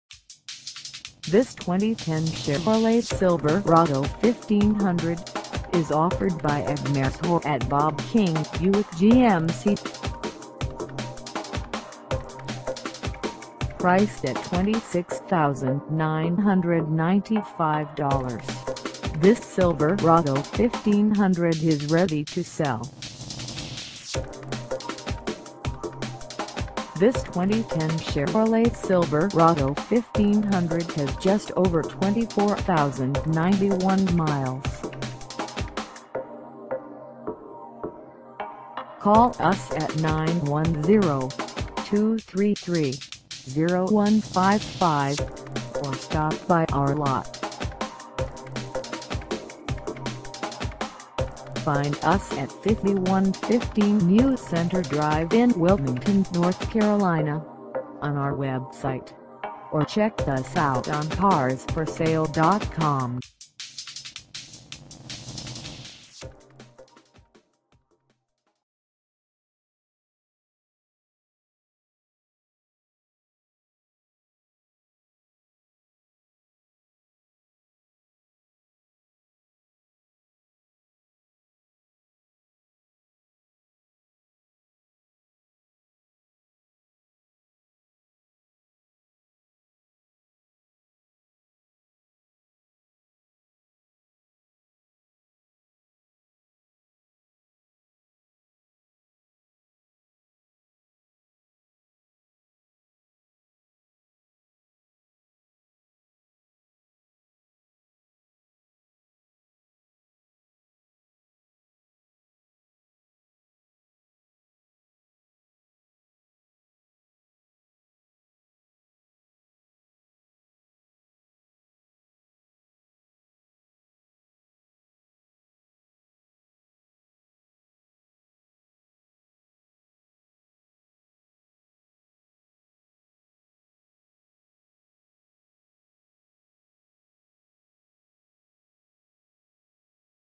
2010 Chevrolet Silverado 1500 LS - for sale in Wilmington, NC 28403
Ed Meshaw at Bob King Buick GMC
5115 NEW CENTRE DR

Come test dirve this 2010 Chevrolet Silverado 1500 LS for sale in Wilmington, NC.